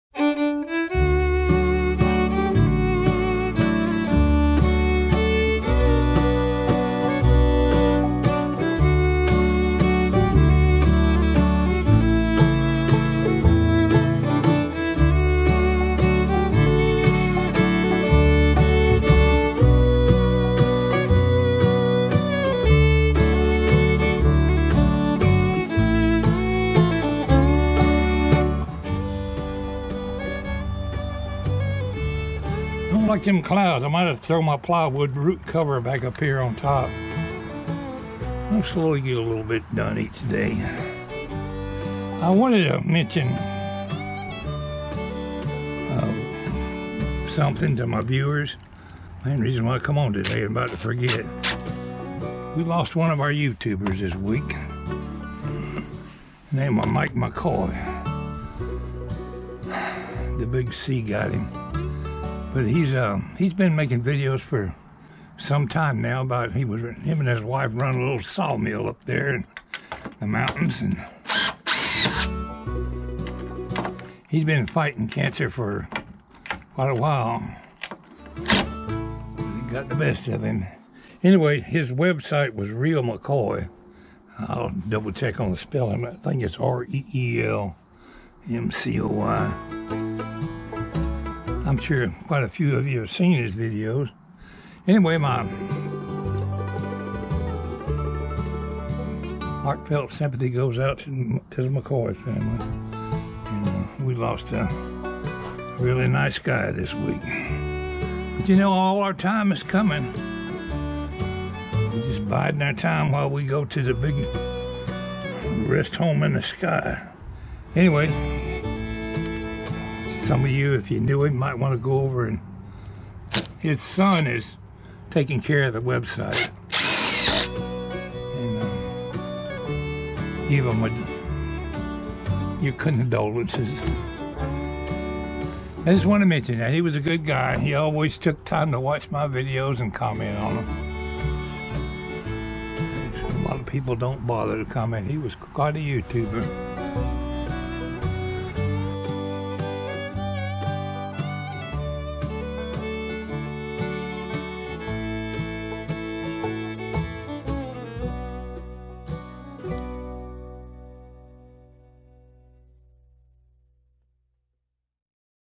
Pwalpar VLOG & The Late Mike McCoy 9 21 2020
I take A moment to remember A fellow YouTuber Mike McCoy